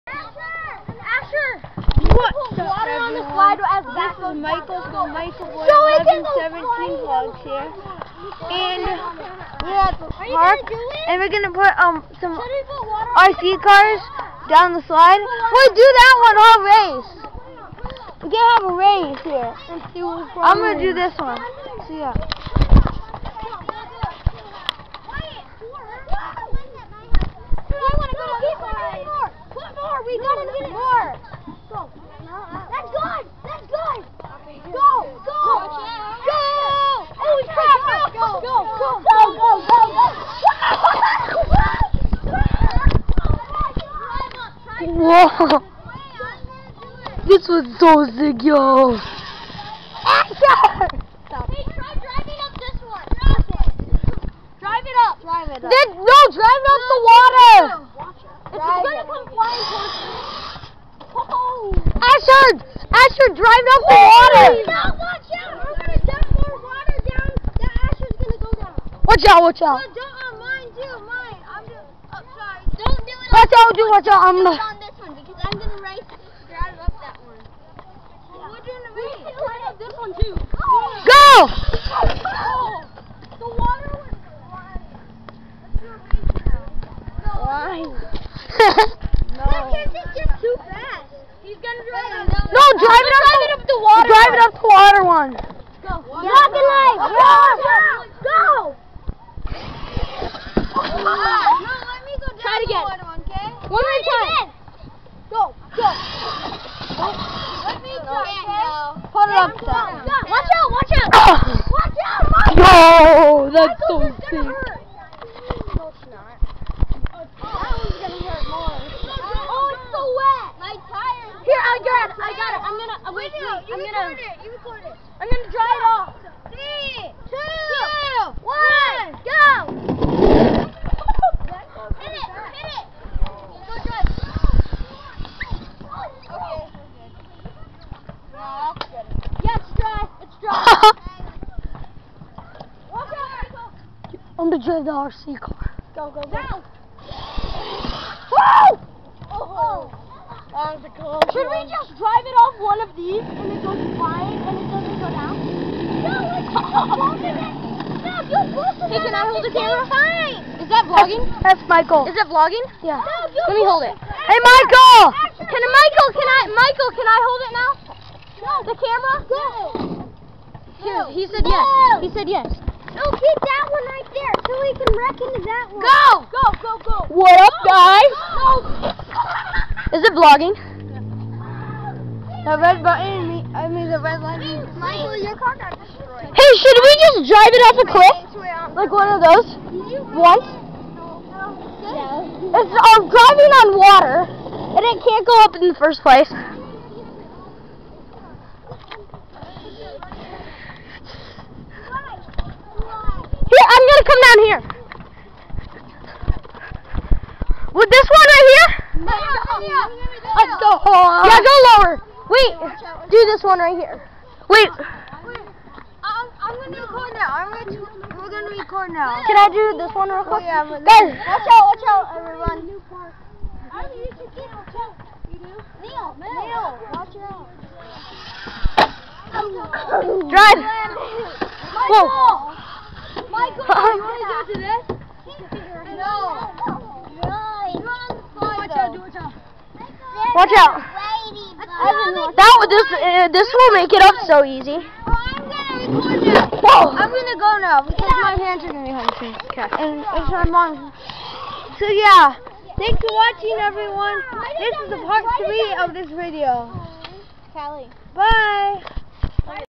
rc car at the park (Part 3) [72]
Michael from MichaelBoy1117Vlogs did a part 3 rc car video at the park.